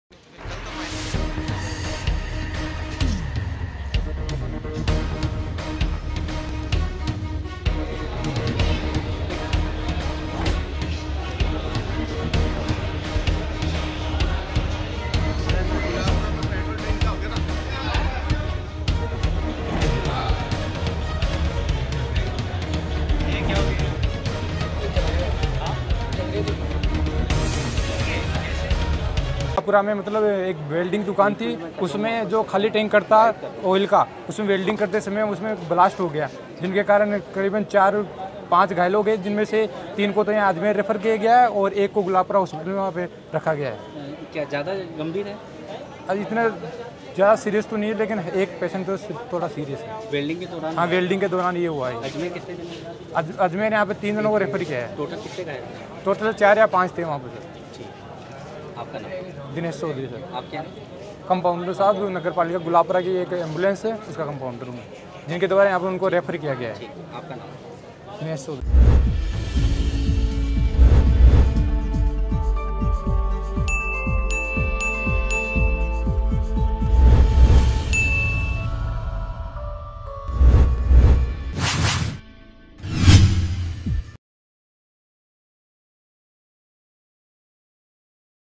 AJMER NEWS | ऑयल के खाली टैंकर में वेल्डिंग के समय हुआ हादसा | MTTV INDIA
ऑयल के खाली टैंकर में वेल्डिंग के समय हुआ हादसा
हादसे में चार - पांच व्यक्ति हुए घायल
घायलों का इलाज जेएलएन हॉस्पिटल में जारी
हादसा गुलाबपुरा के एक निजी वेल्डिंग दुकान पर हुआ